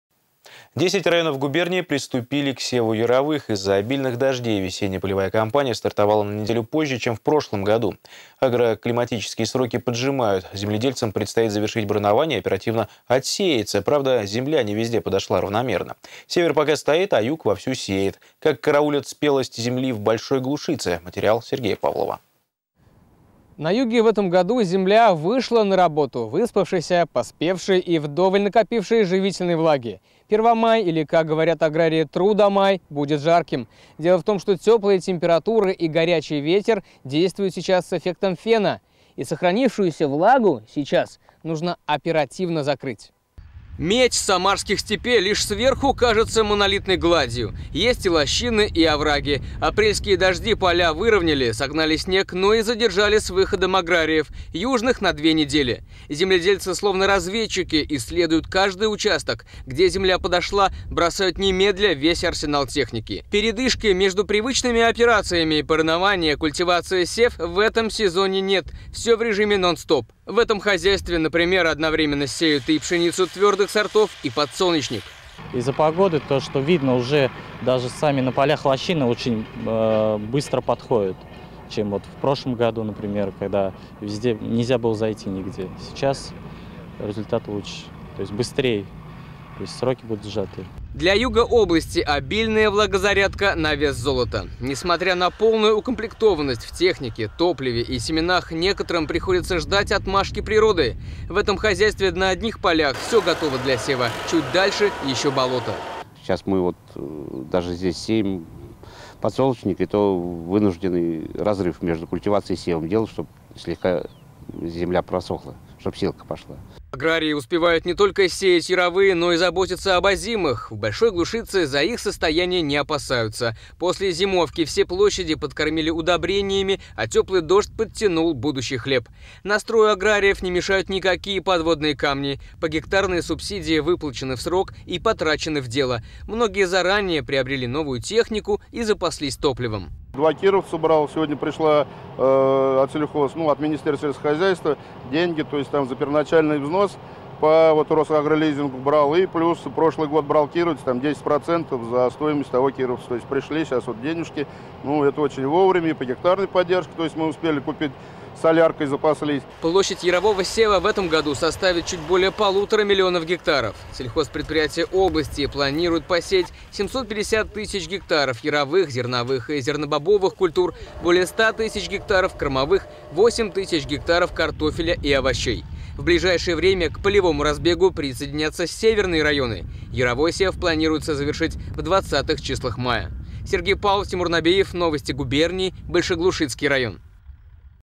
В Самарской области начался массовый сев яровых
10 районов губернии приступили к севу яровых. Из-за обильных дождей весенне-полевая кампания стартовала на неделю позже, чем в прошлом году.

На юге в этом году земля вышла на работу выспавшейся, поспевшей и вдоволь накопившей живительной влаги. Первомай или, как говорят аграрии, трудомай, будет жарким. Дело в том, что теплые температуры и горячий ветер действуют сейчас с эффектом фена. И сохранившуюся влагу нужно оперативно закрыть.

Медь самарских степей лишь сверху кажется монолитной гладью. Есть и лощины, и овраги. Апрельские дожди поля выровняли, согнали снег, но и задержали с выходом аграриев. Южных - на две недели. Земледельцы словно разведчики: исследуют каждый участок, где земля подошла - бросают немедля весь арсенал техники. Передышки между привычными операциями - боронование, культивация, сев - в этом сезоне нет, все в режиме нон-стоп. В этом хозяйстве, например, одновременно сеют и пшеницу твердых сортов, и подсолнечник.

Для юга области обильная влагозарядка - на вес золота. Несмотря на полною укомплектованность в технике, топливе и семенах, некоторым приходится ждать отмашки природы. В этом хозяйстве на одних полях все готово для сева, чуть дальше - еще болото.

Аграрии успевают не только сеять яровые, но и заботиться об озимых. В Большой Глушице за их состояние не опасаются: после зимовки все площади подкормили удобрениями, а теплый дождь подтянул будущий хлеб. Настрою аграриев не мешают никакие подводные камни: погектарные субсидии выплачены в срок и потрачены в дело: многие заранее приобрели новую технику и запаслись топливом.

Площадь ярового сева в этом году составит чуть более 1,5 млн гектаров. Сельхозпредприятия области планируют посеять 750 тысяч гектаров яровых зерновых и зернобобовых культур, более 100 тысяч гектаров кормовых, 8 тысяч гектаров картофеля и овощей. В ближайшее время к полевому разбегу присоединятся северные районы. Яровой сев планируется завершить в 20-х числах мая.